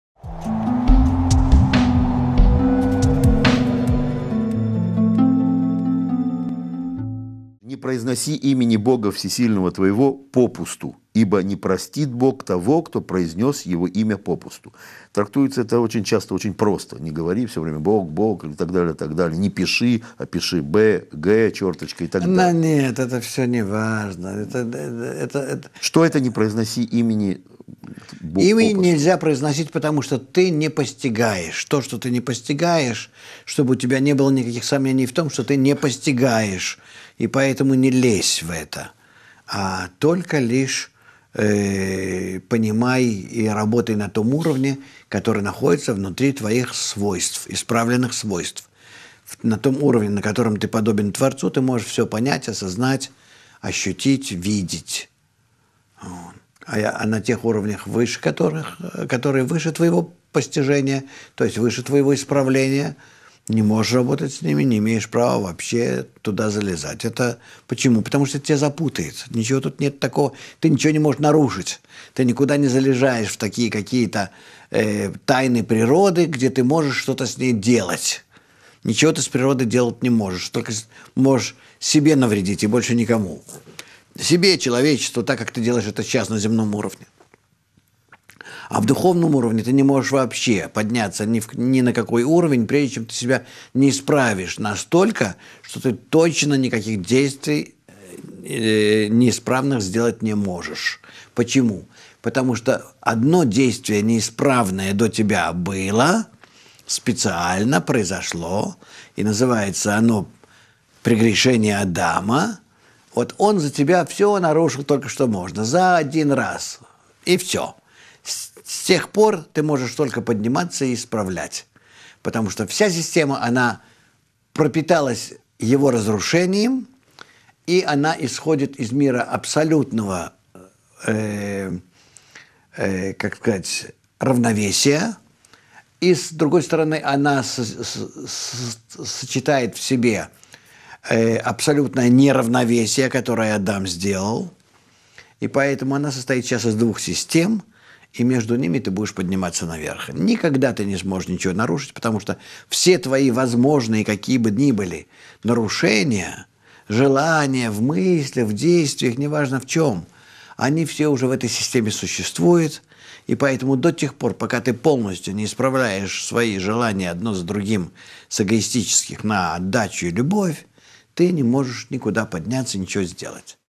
Не произности имени Бога попусту. Ваэтханан, передача 4
На духовном уровне исключены неправильные действия. За всю историю человечества произошло всего одно такое действие, которое было сделано специально и называется «прегрешением Адама».